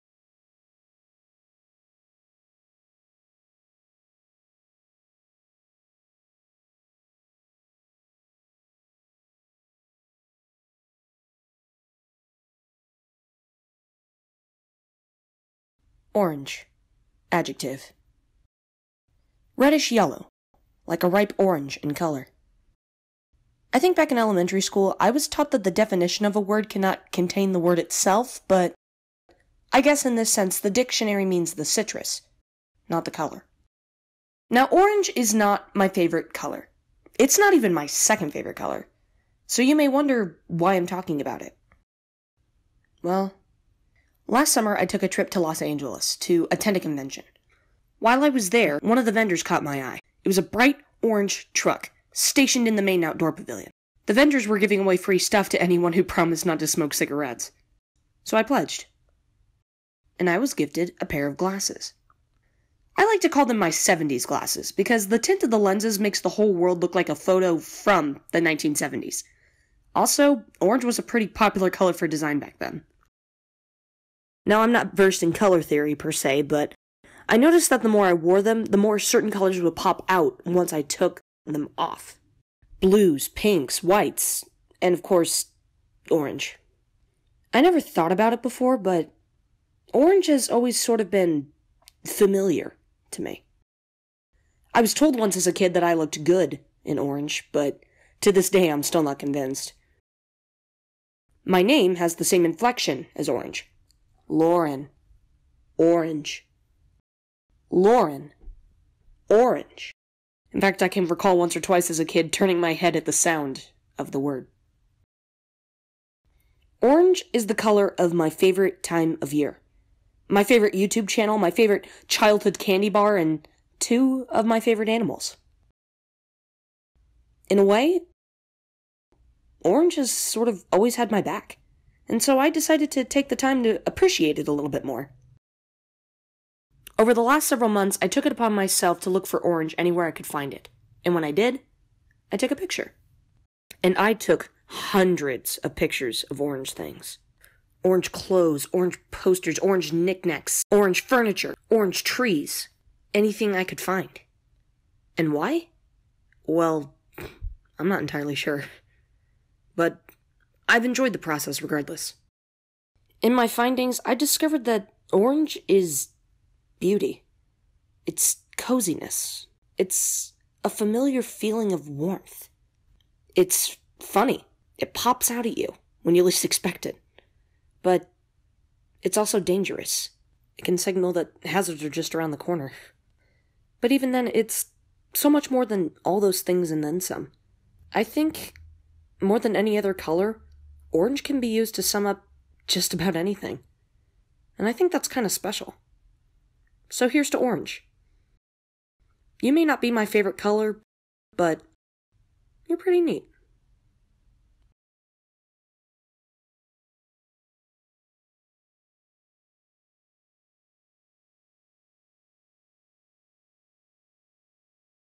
Short Film: Orange
This week, I share another short film of mine from school: "Orange."